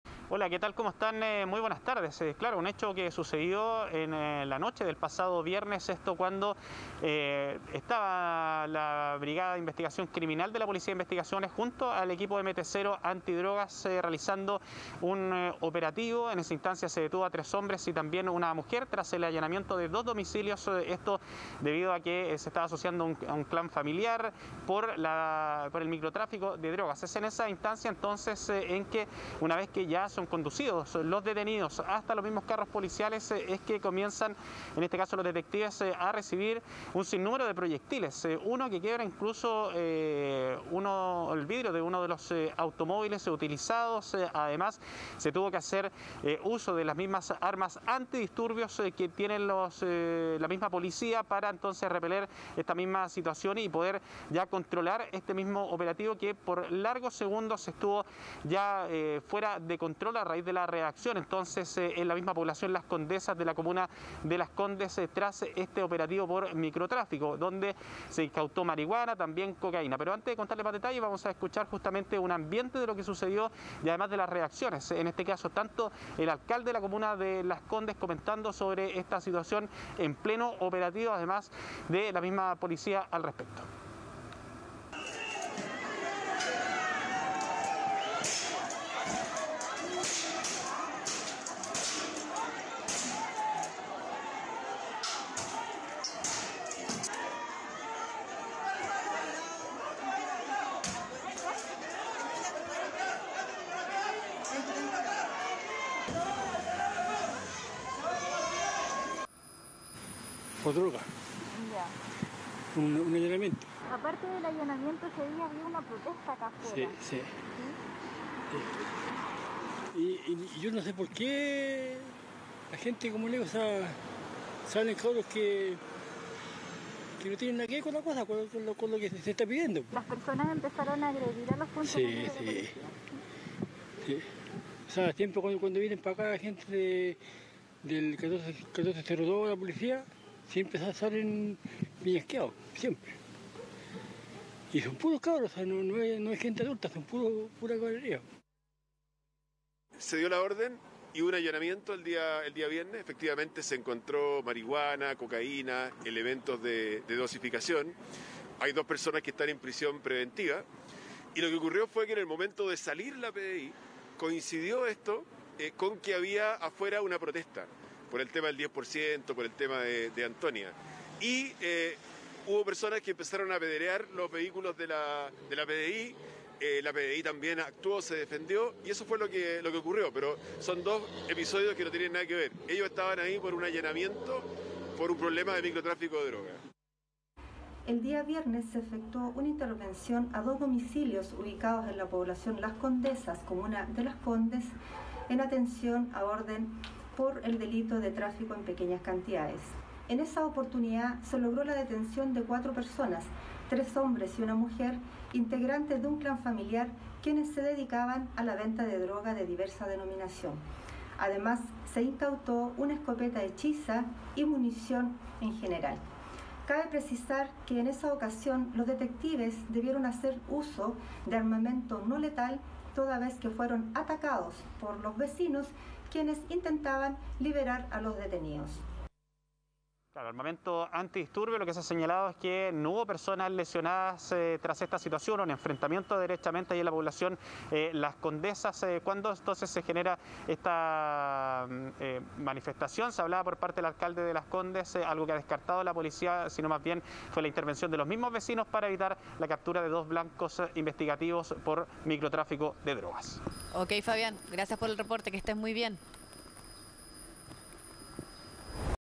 Funcionarios de la PDI fueron atacados en medio de operativo antidrogas en Las Condes
Justo cuando cuatro personas eran detenidas tras ser investigadas por microtráfico de marihuana y cocaína, los miembros de la Brigada de Investigaciones Criminales y del MT-Cero recibieron proyectiles lanzados por vecinos del lugar.